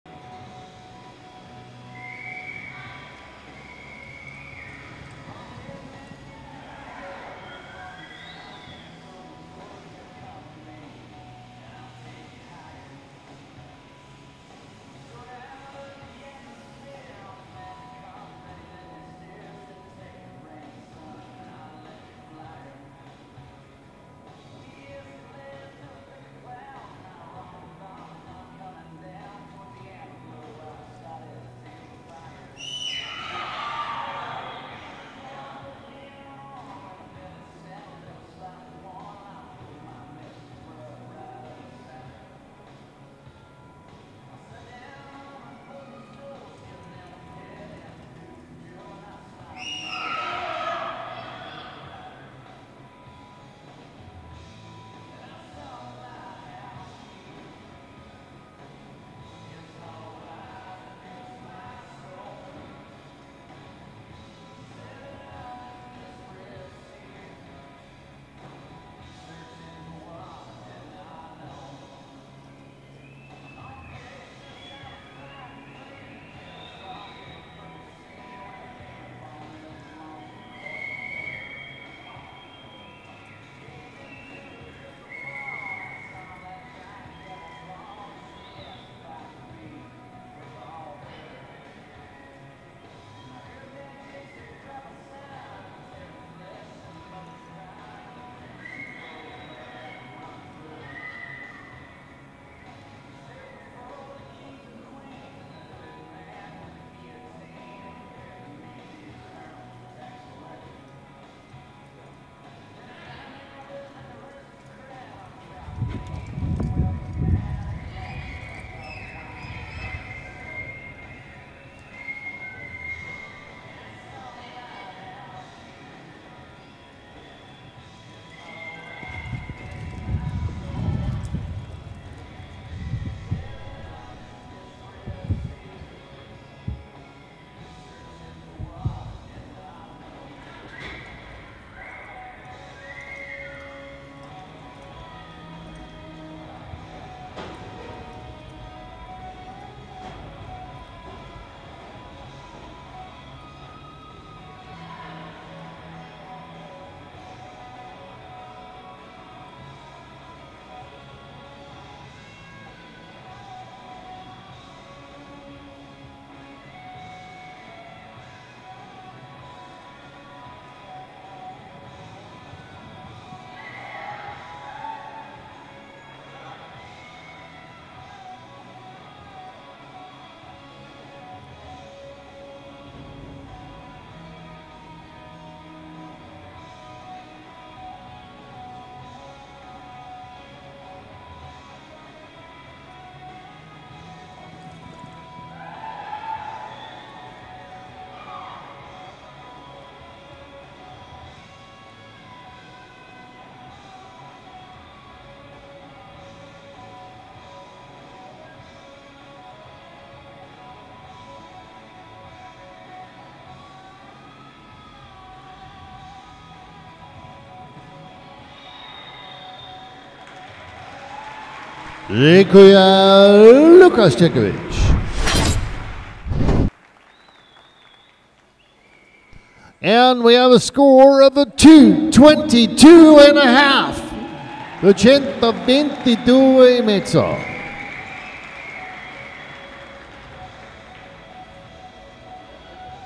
Futurity 2024 - LUKASZ CZECHOWICZ & CSG BERLIN score 222,5
LUKASZ CZECHOWICZ & CSG BERLIN owner DOR COHEN score 222,5 (75,5 74,5 73,5 74 74) - IRHA-IRHBA-NRHA FUTURITY 3 YRS OLD OPEN LEVEL 4 Go Round Section Top Score - Futurity IRHA-IRHBA-NRHA-FISE 2024 - credits Bonaga Communication